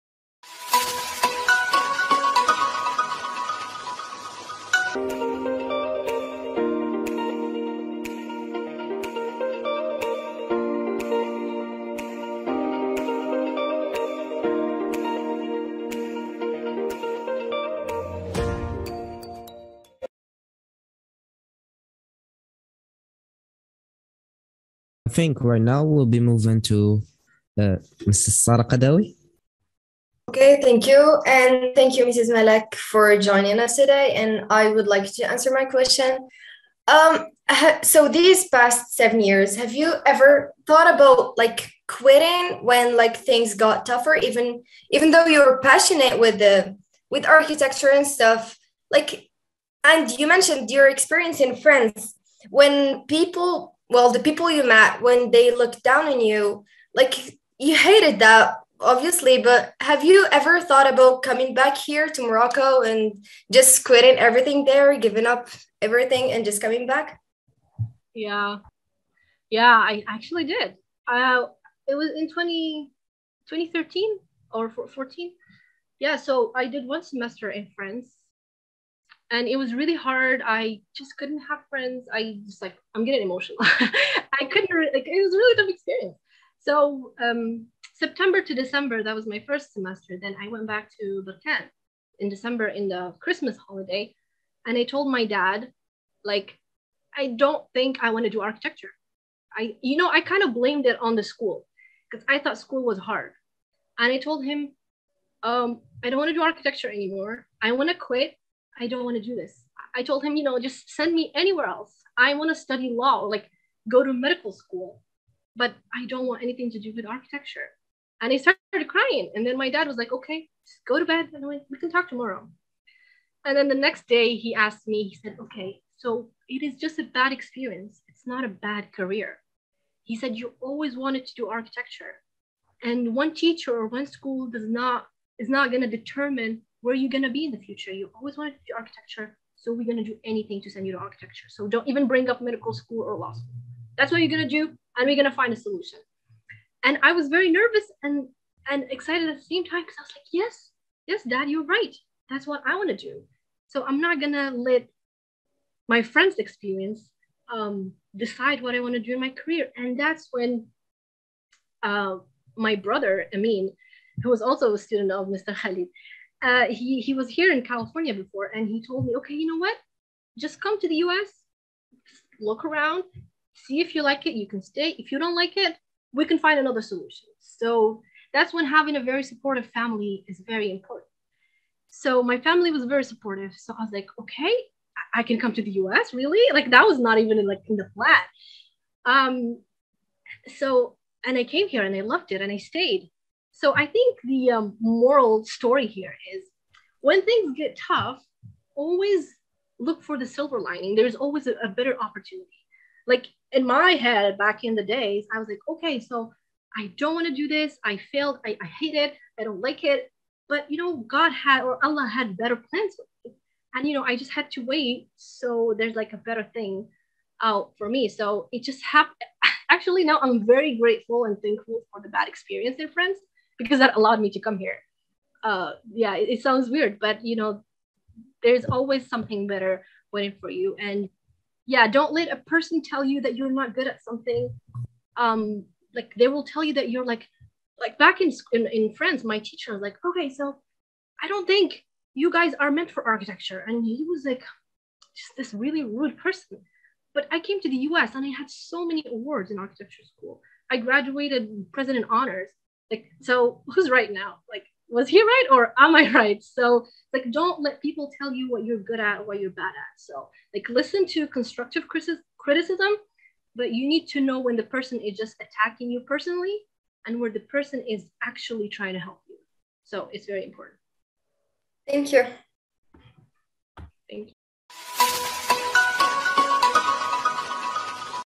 High Up Academy For English and Leadership: High Up Elite E-meets Malak Bellajdel (2nd Episode)
High Up students invite great former OKE or High Up students to get inspired and learn from their experience. This month, we meet Malak Bellajdel, an architect residing in San Francisco. As the interview is very interesting, we will have it in 3 episodes.